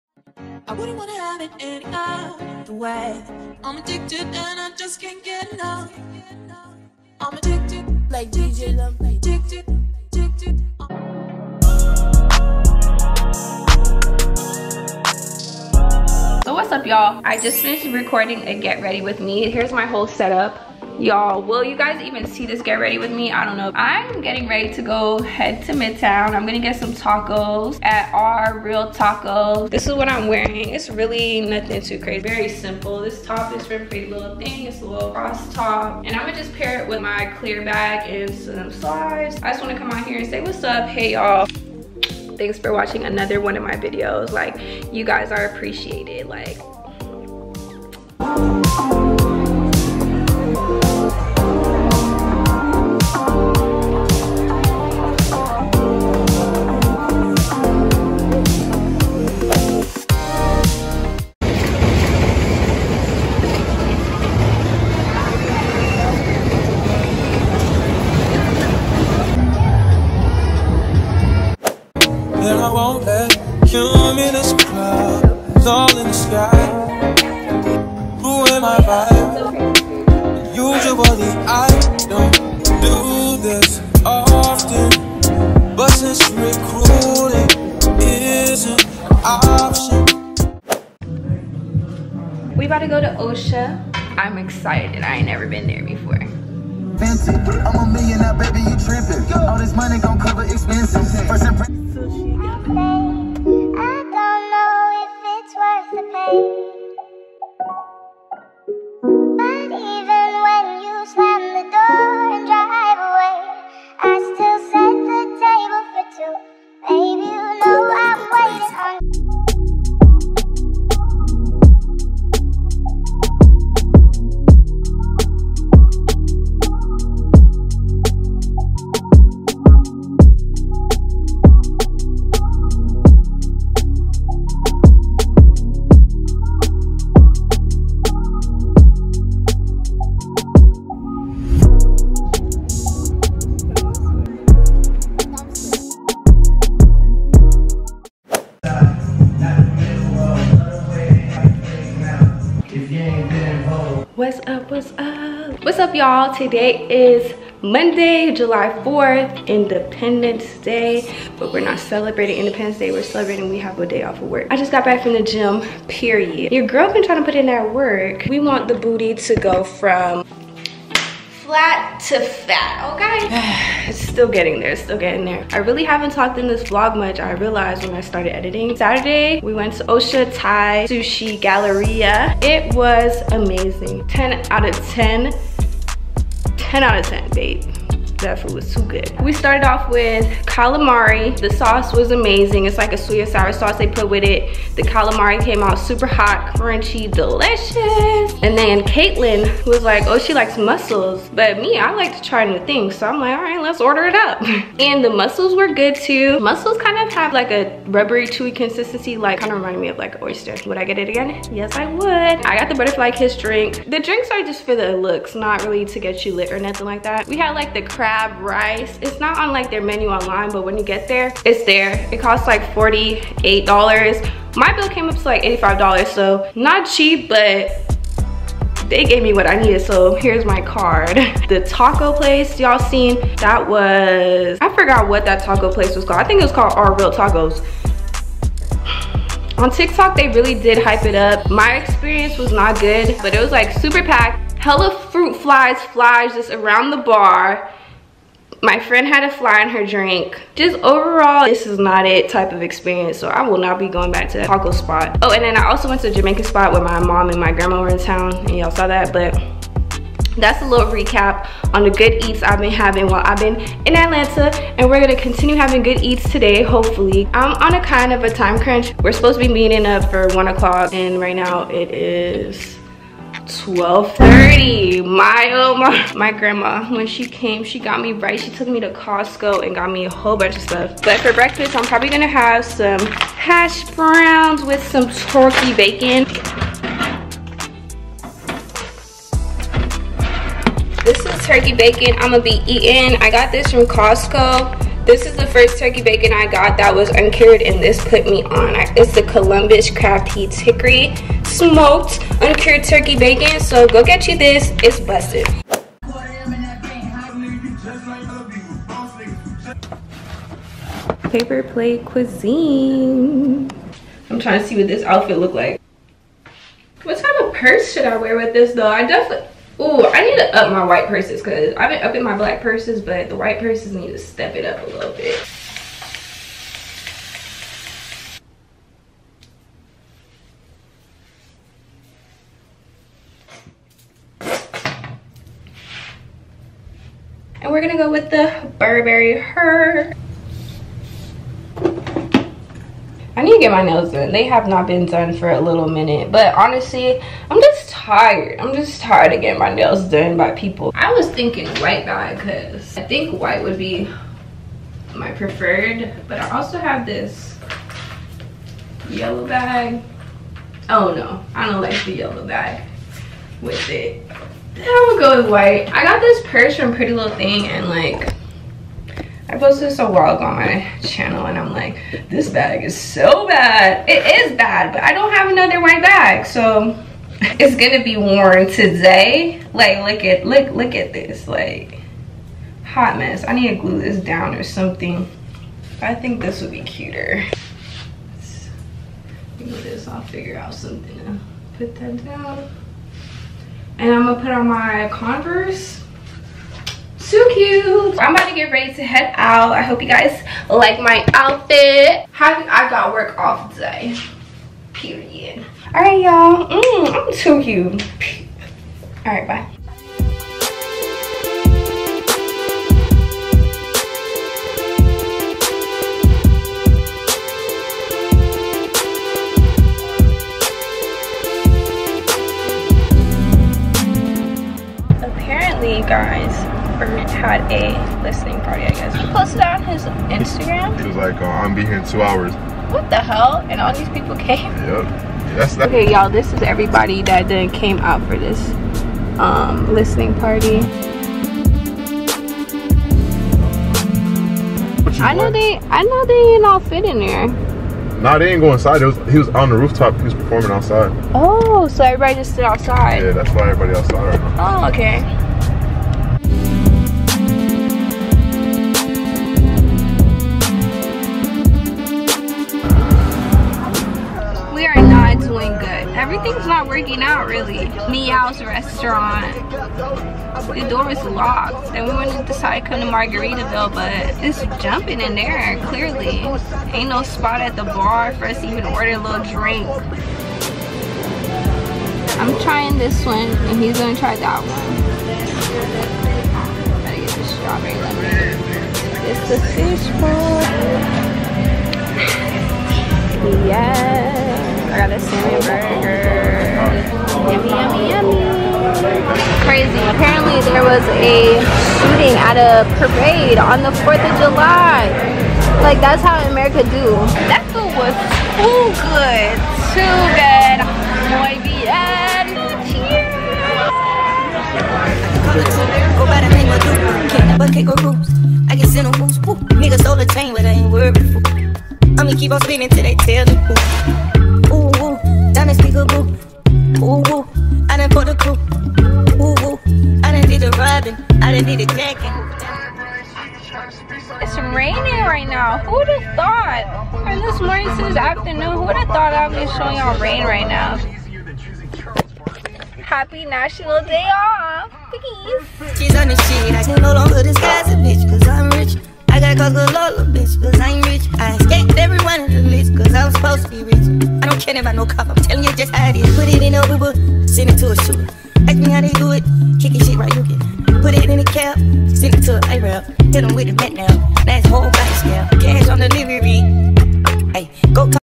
summer VLOG | eating out, met brent faiyaz?, lit weekend + more fun
WATCH IN HIGHEST QUALITY!

we are trying new restaurants all summer! plenty of good eats in the Atlanta area in this video. I visited RReal Tacos in midtown, Osha Thai & Sushi Galleria, Magaritaville, and Big Kahuna. I even ended up at a listening party for Brent Faiyaz and caught the end. Battery Atlanta was a fun vibe and so many restaurants to chose from. I hope you all enjoy the vlog! Let me know down below if you do.

FAQS :)
`age - 23
`camera - canon gx7 markii

♡ social media | instagram & twitter | @cuhshawndray